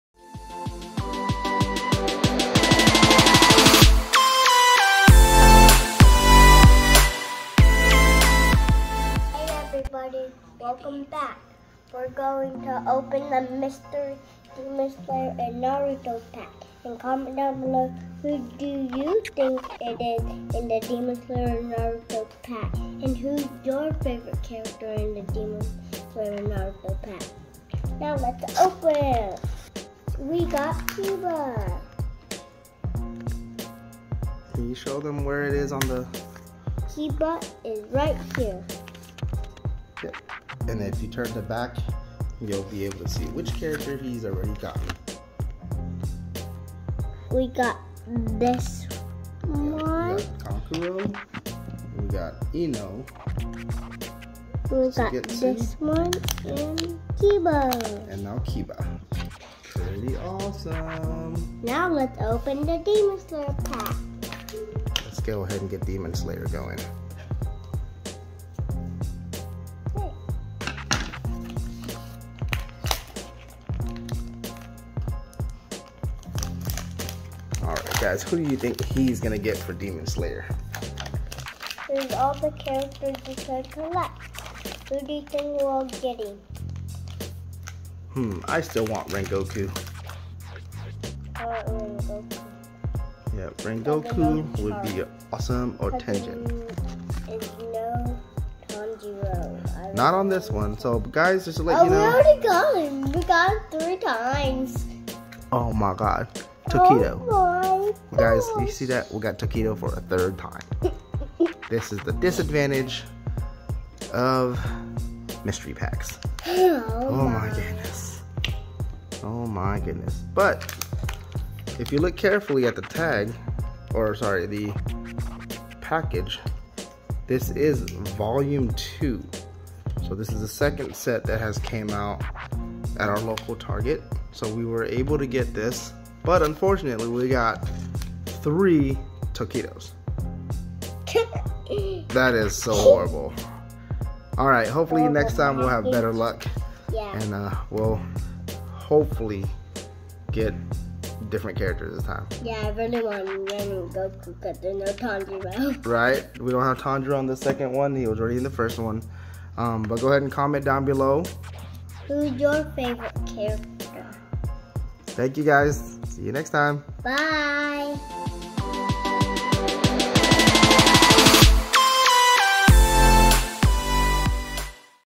Naruto and Demon Slayer mystery packs!!
hey everyone just a quick short video here on the Naruto and Demon Slayer mystery packs.

Today we're going to have a quick rundown of what we got and how it adds to our collection.

Also we're going show you the downfalls of mystery packs sometimes you get things you want and other times you don't thank you!!